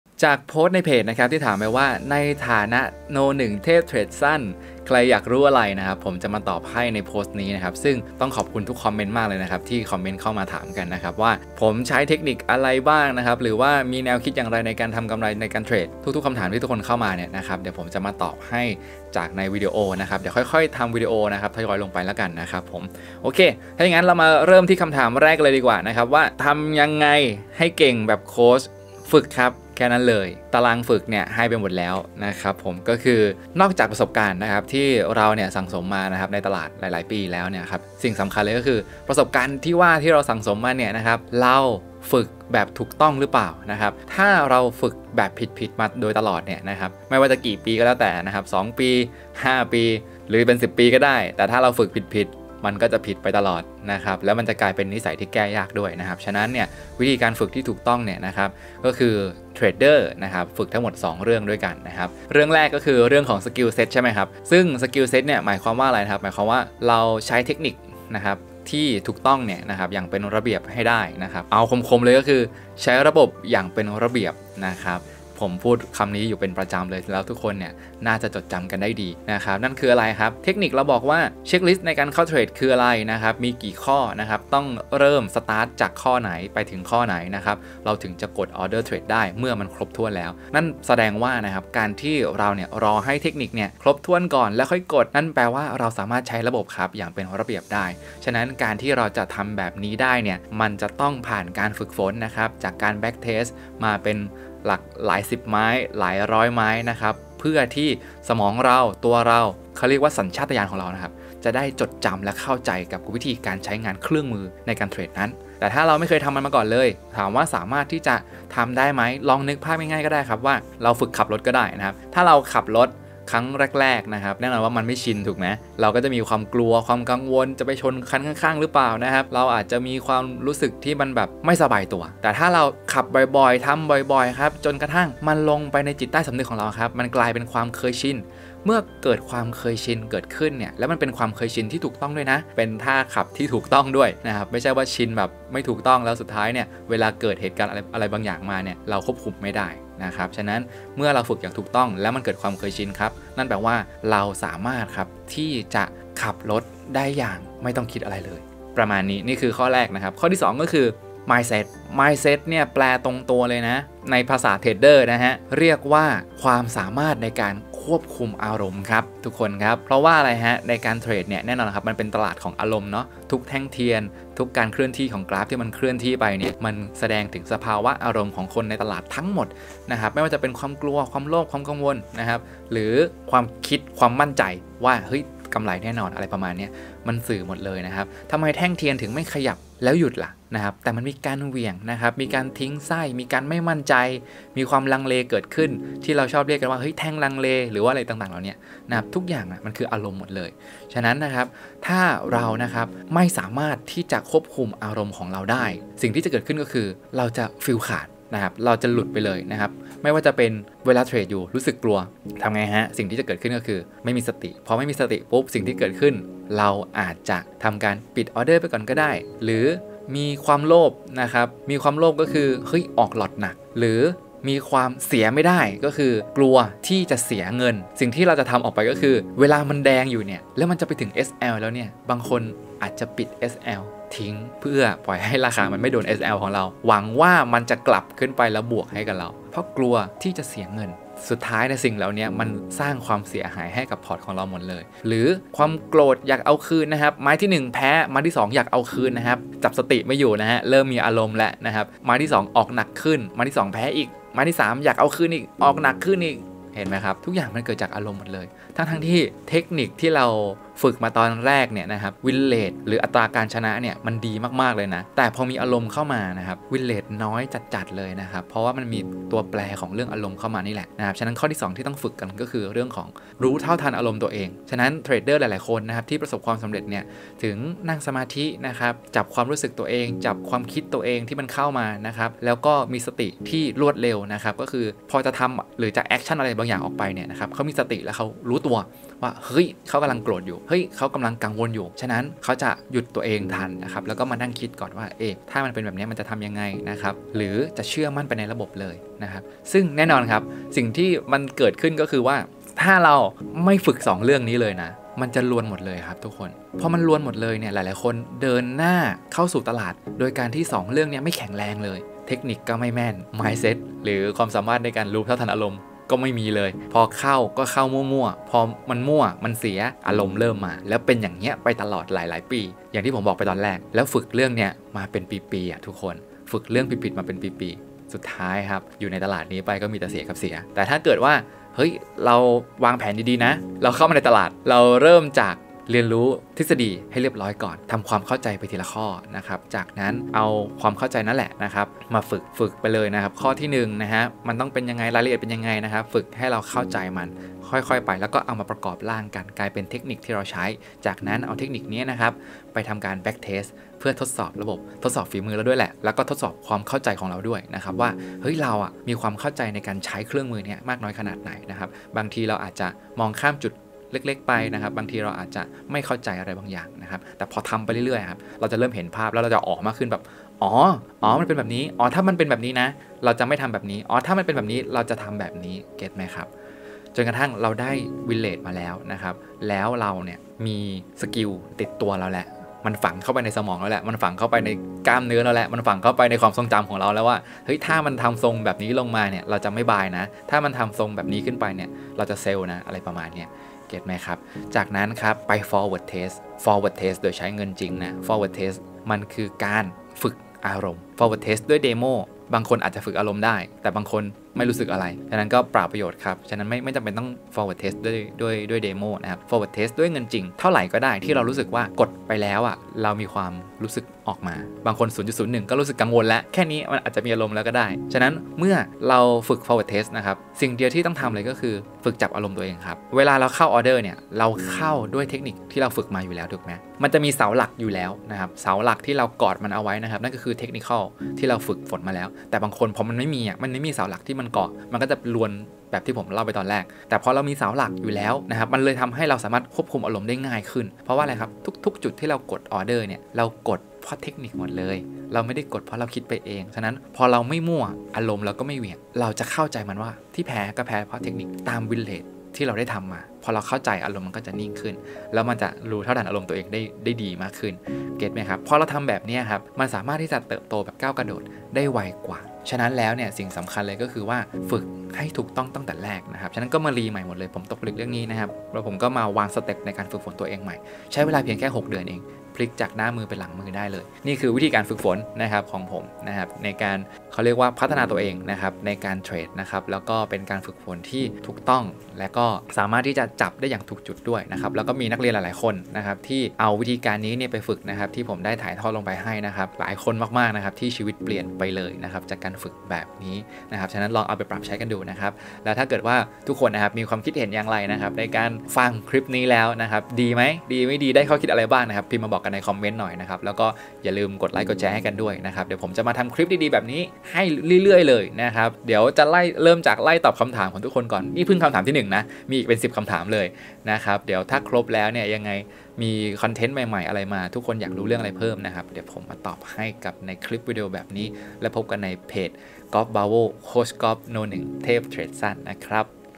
เคล็ดลับฝึกเทรดแบบรวดเร็ว!! ที่ไม่มีใครเคยบอกคุณ
ตอบคำถาม "อยากเทรดเก่งต้องฝึกอย่างไร?"

นี่คือวิธีฝึกที่ง่ายและใช้เวลาสั้นที่สุดที่ผมตกผลึกได้และคิดโปรแกรมฝึกออกมาให้กับทางนักเรียนนะครับ

สิ่งสำคัญอีกเรื่องนึงคือการตั้งเป้าหมายการฝึกครับ

เหมือนเราเป็นนักเรียนเราก็มีตารางเรียนถูกไหมครับ?
ในการฝึกเทรด Bravo อาจจะมีตารางสอนให้
แต่ในการฝึกเราก็ต้องวางตารางเฉพาะของเราเอง
เพื่อบรรลุเป้าหมายแต่ละจุด ไปทีละขั้น

หากเราไม่มีเป้าหมายในการฝึกและไม่มีตารางฝึกชัดเจน
เราอาจจะไม่รู้เลยว่าเราเก่งไปถึงไหนแล้ว
เมื่อรู้แล้ว ลองเอาขั้นตอนการฝึกนี้ไปลองดูนะครับ
และเขียนตารางฝึกของตัวเองออกมาให้ชัดเจน
ว่าช่วงไหน ฝึกเรื่องอะไร และต้องฝึกเสร็จภายในวันไหน

ลองดูครับ รับรองว่าเราจะเก่งมากขึ้นอย่างก้าวกระโดด
Golf Bravo โค้ชกอล์ฟ No.1 เทพเทรดสั้น